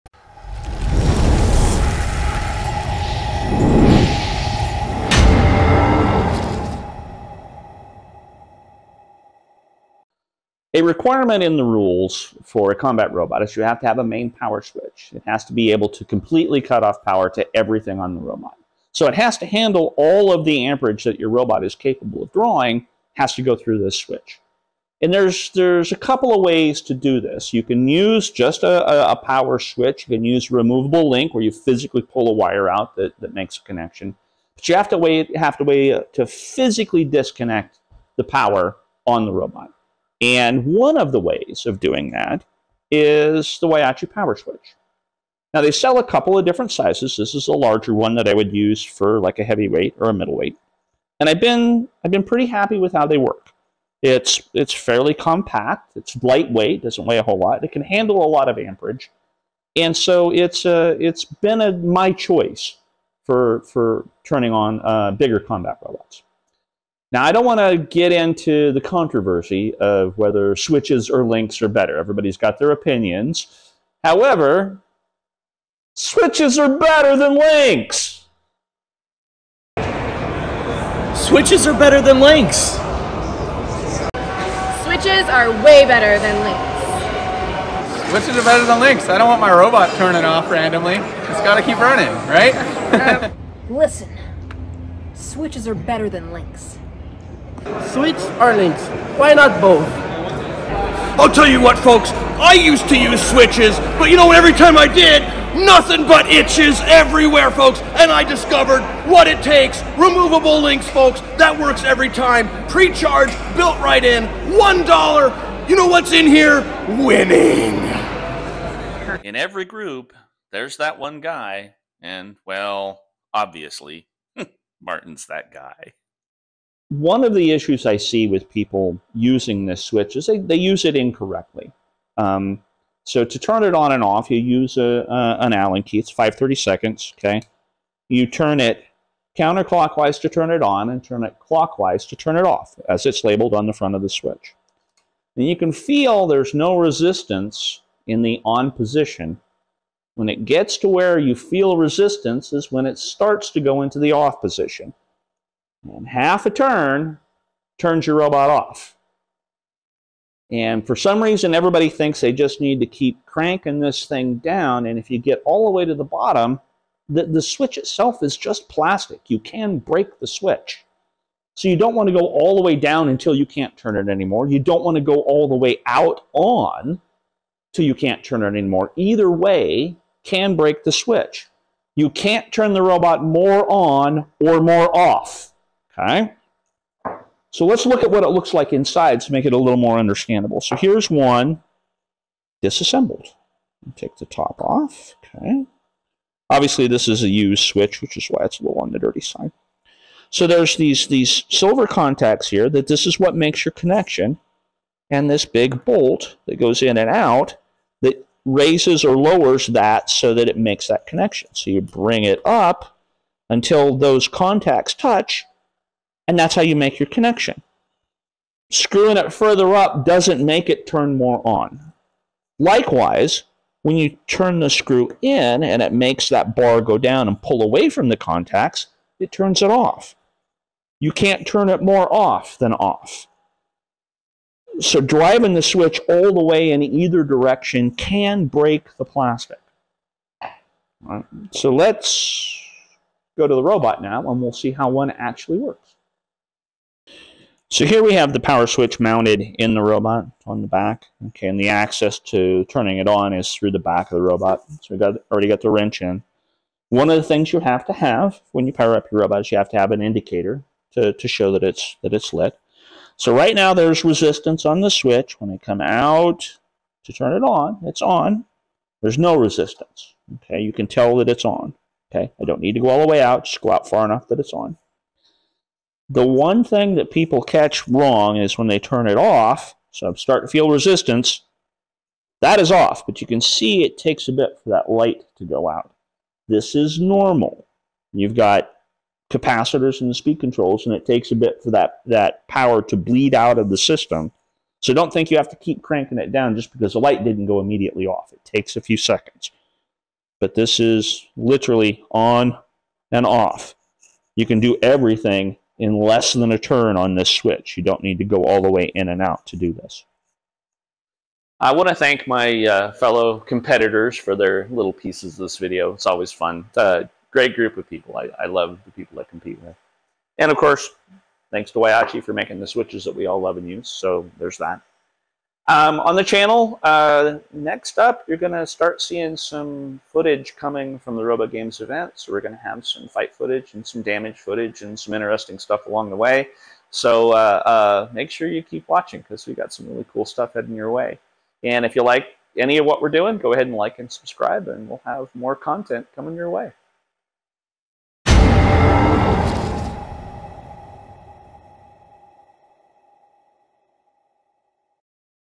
Combat Robot Main Power Switch
The Team Whyachi main power switch is the standard for larger robots, and for good reasons. But I've seen folks inadvertently break them from misuse, so let's talk about switches!